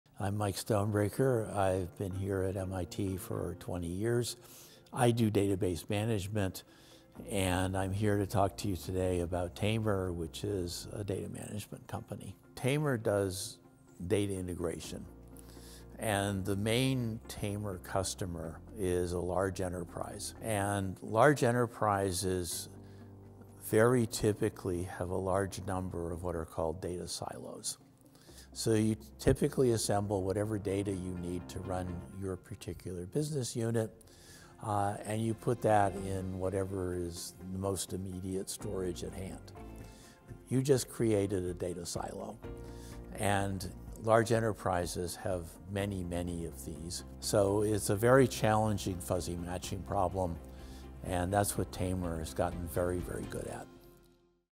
See how Turing Award winning Professor Mike Stonebraker spun his research out into a company
Find the full story of CSAIL Professor Mike Stonebraker's company Tamr